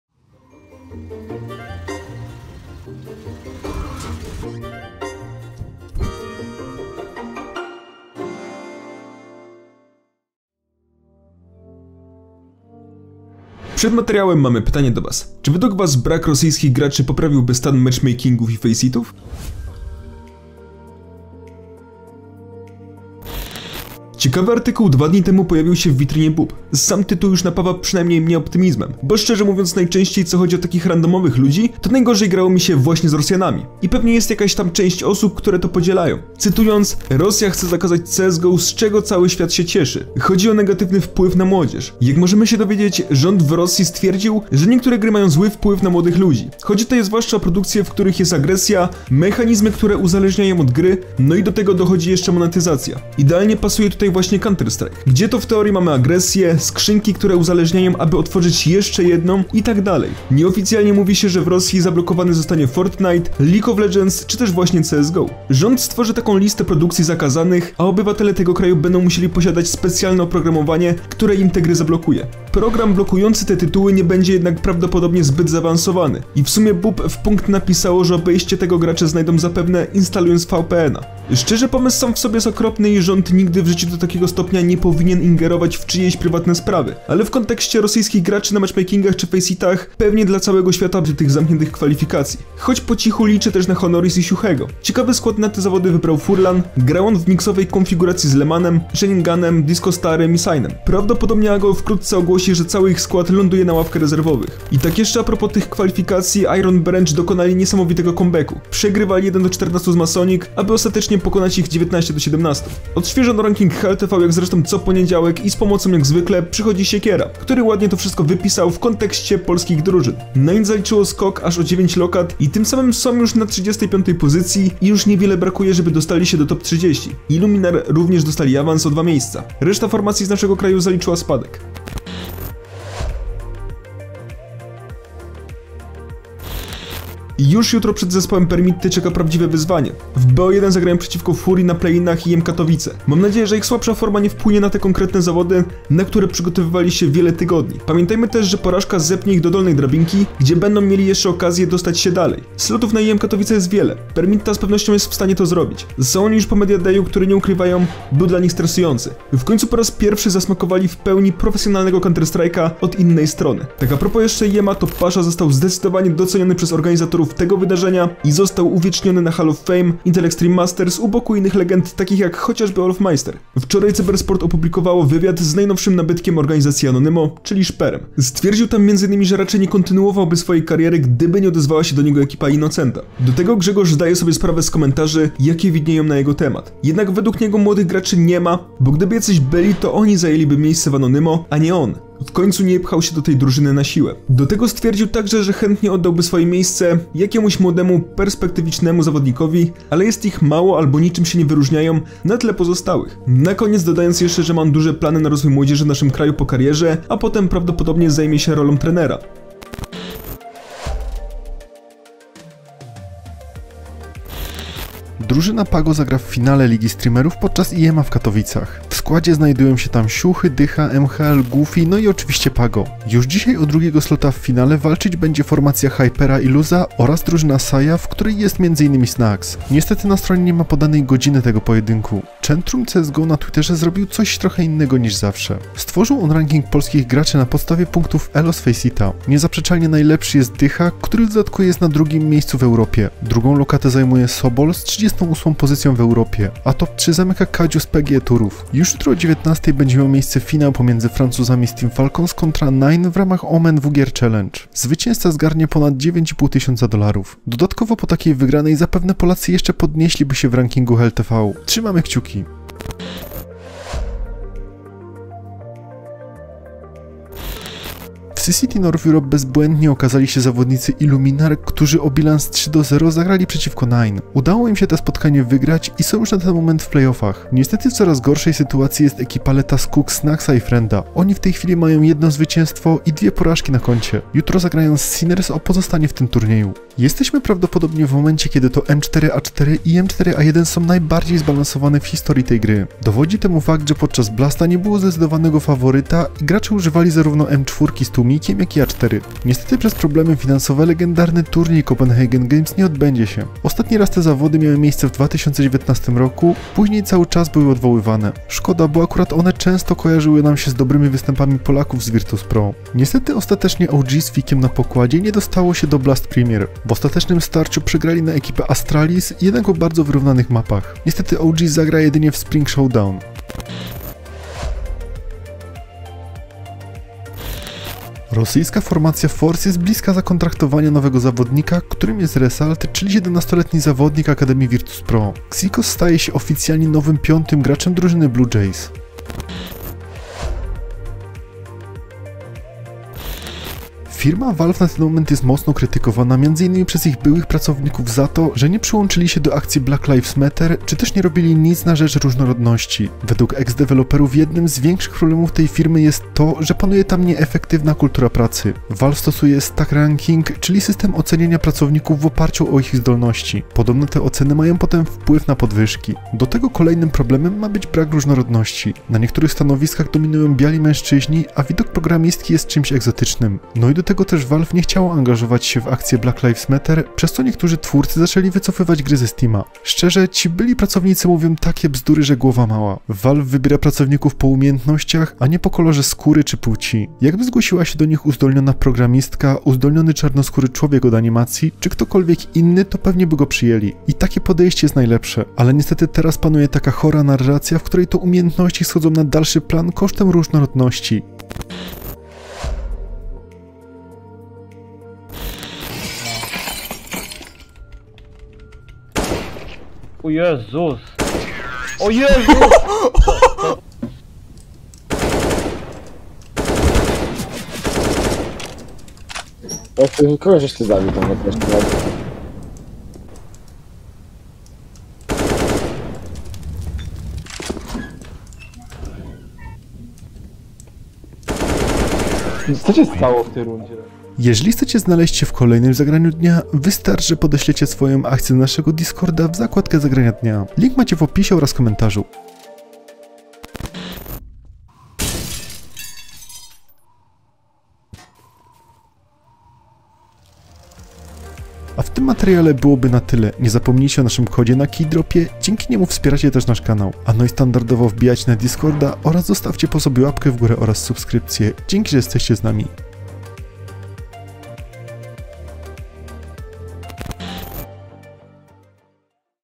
KONIEC ROSYJSKICH GRACZY W CS:GO?! 🔥 SZPERO ODPOWIADA NA HEJT! PASHA DOCENIONY NA HALL OF FAME!
Wstęp - 0:00​​
Koniec rosyjskich graczy CS:GO! Nowy projekt w Rosji! - 0:19
 - 1:31
Wielka szansa Polaków z 9INE! Polacy walczą o IEM Brazil! - 2:04
Pasha na Hall of Fame! Szpero odpowiada na Hejt! - 3:22
Skład PAGO w finale na IEMa! Ranking polskich graczy FACEITA! - 4:40
Najlepszy balans M4A4 i M4A1? Problemy legendarnego turnieju! - 5:33
Strefa Transferowa - 6:34
Strefa Gamingu - 6:50
Zagranie Dnia - 7:51
Zakończenie - 8:34